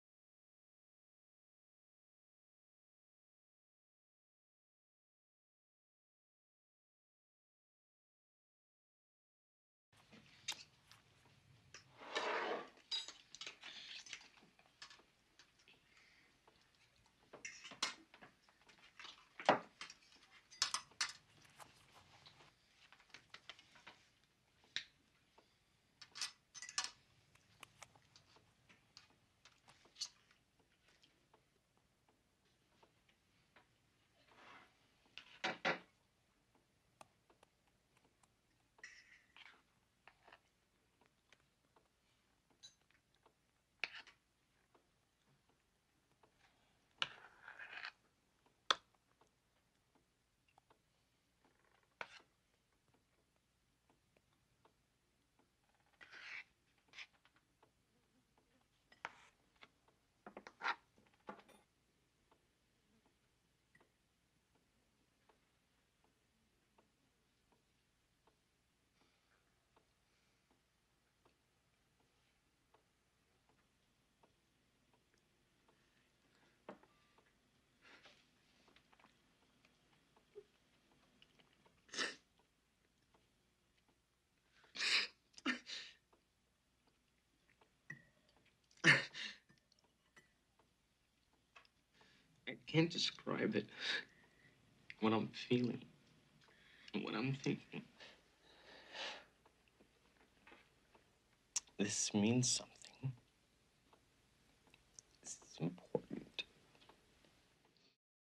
Why the mashed potato scene is my favorite scene from "Close Encounters of the Third Kind"
So... I watched Close Encounters for the first time in over 15 years and realized why I like the mashed potato scene so much...